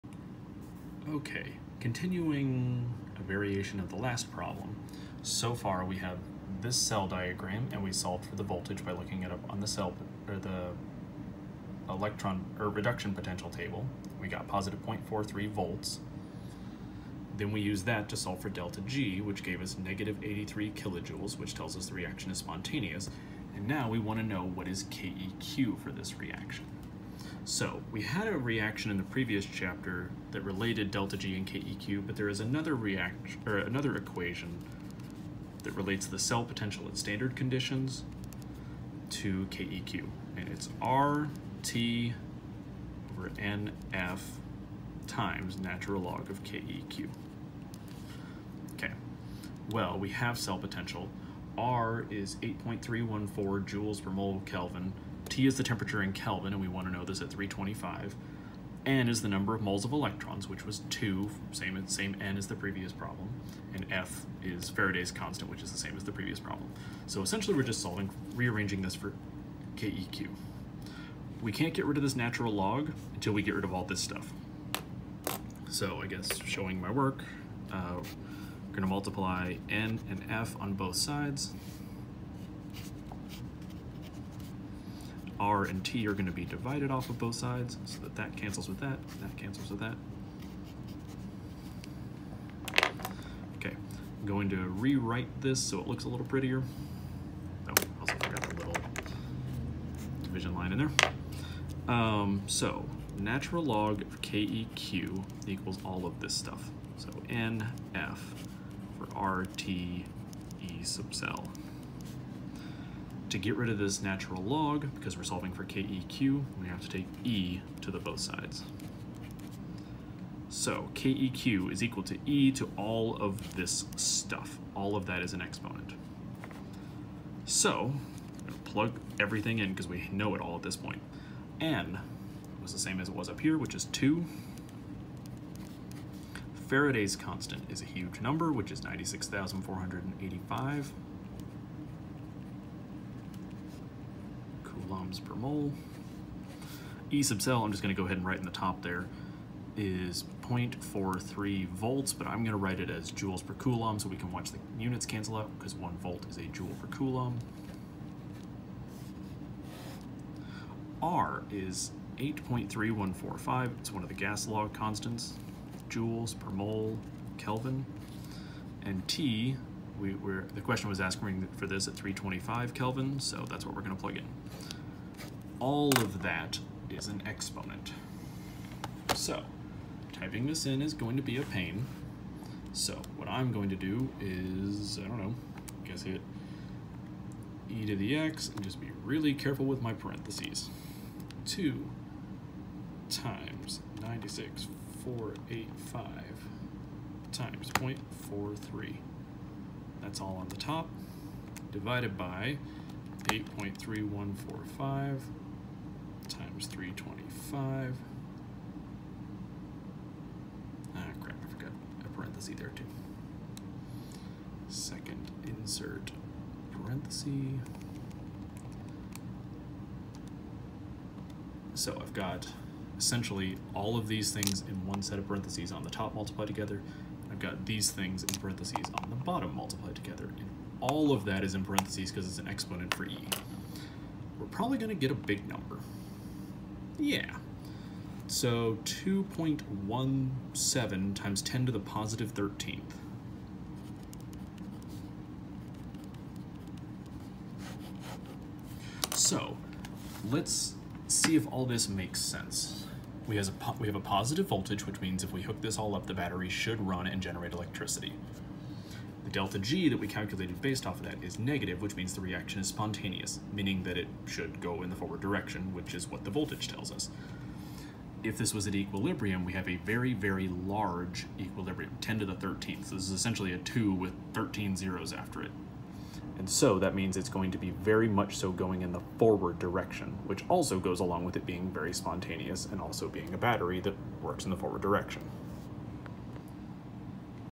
Calculating Keq from cell potential
Using cell potential (voltage) to determine the equilibrium constant for a cell diagram.